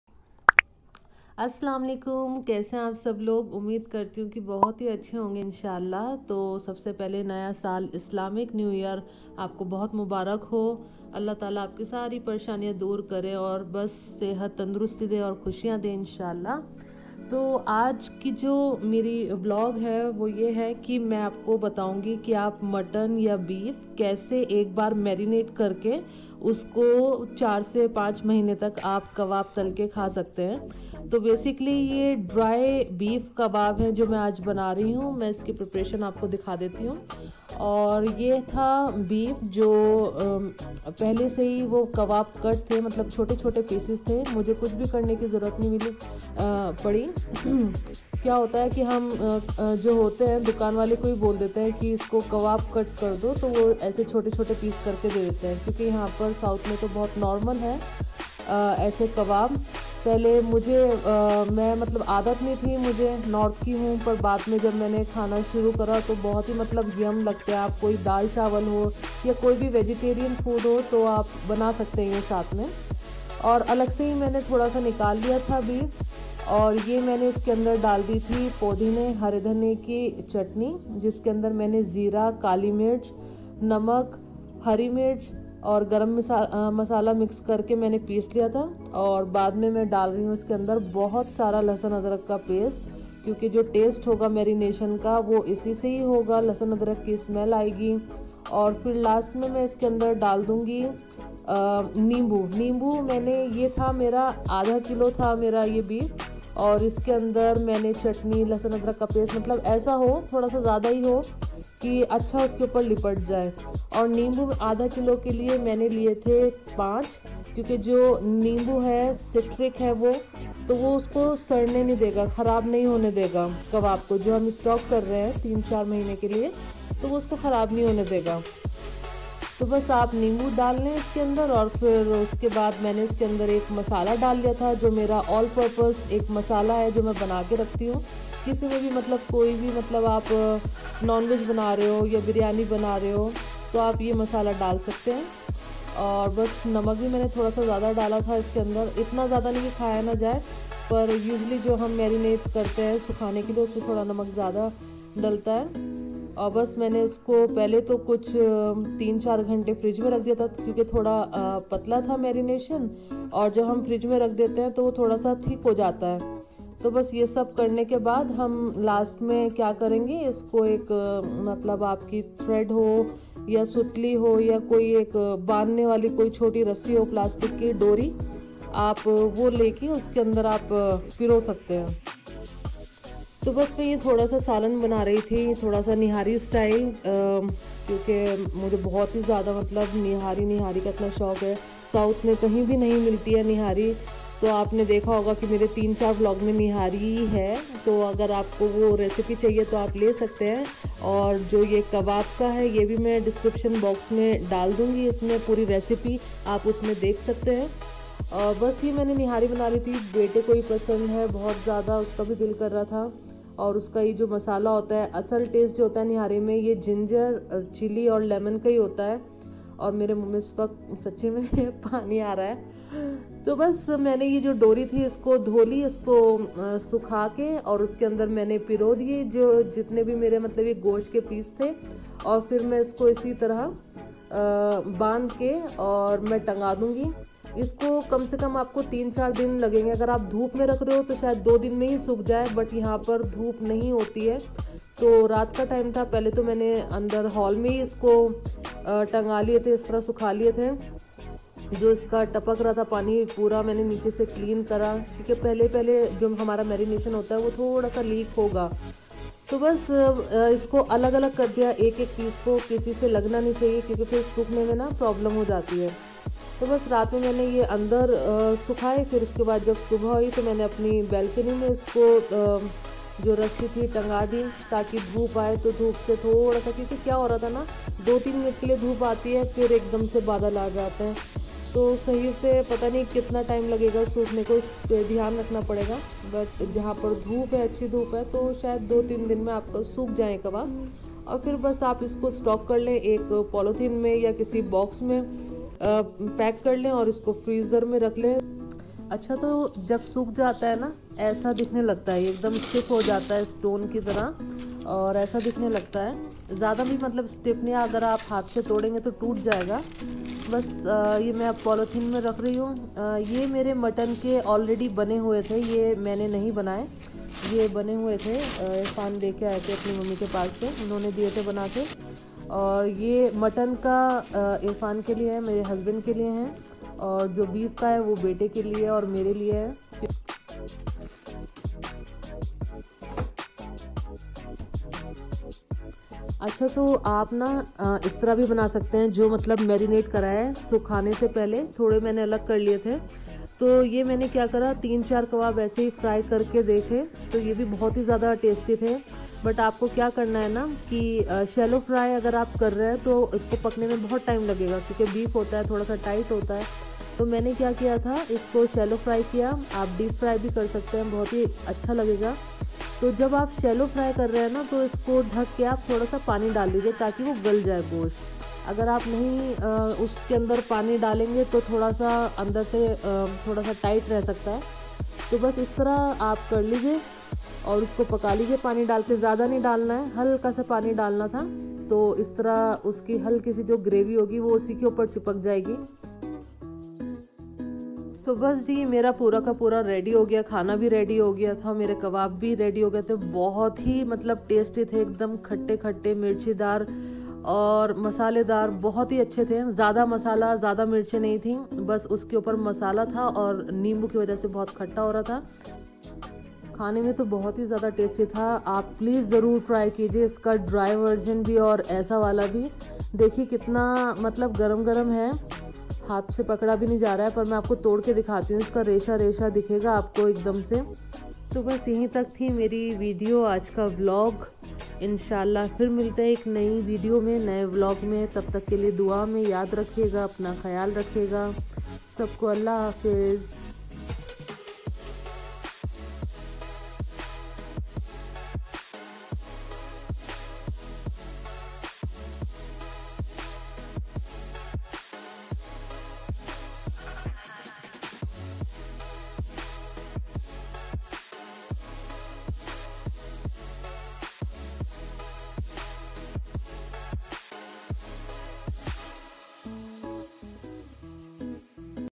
Beef ke khatay dry kabab || merinate once stock it for 3 months
beef dry kabab
for marination you need meat of your choice like mutton or beef
cut it into small pieces wash it off nicely pet dry with cotton cloth
pudina dhaniya jeera lahsun adrak paste kali mirch namak garam masala
make a paste out of it and marinate beef or mutton
add lemon juice of 5 lemon
keep it in refrigerator for 3 hours
then with the help of needle and thread Pierce the beef and hang it in Sun to dry for 2 to 3 days after that stocke it in freezer...
with vegetarian food on non vegetarian food you can just deep fry your kebab and have fun 😊👍🏼